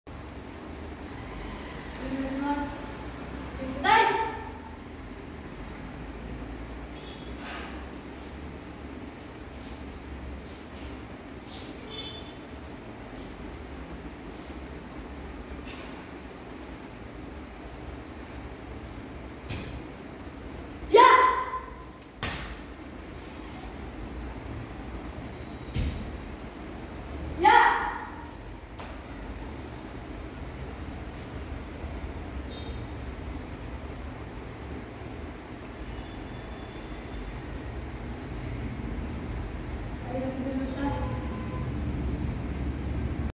Name: Deepshikha KC. Age Category : Under 10 years
Name: Deepshikha KC
Age Category : Under 10 years
Kata: Gekisai dai ichi
Country: Nepal